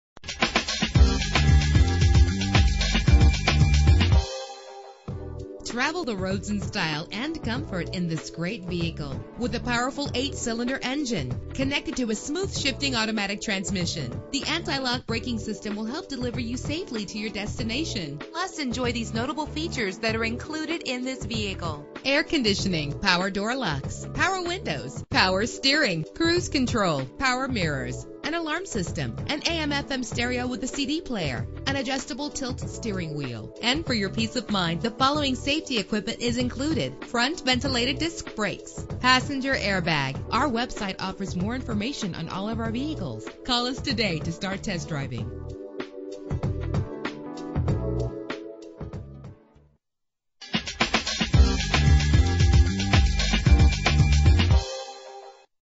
2008 Dodge Ram 1500 - Gainesville GA
Year: 2008
Make: Dodge
Model: Ram 1500
Trim: 160 5
Engine: 5.7 liter 8 cylinder 16 valve
Transmission: Automatic
Color: White
Mileage: 77220
Address:
3115 Frontage Rd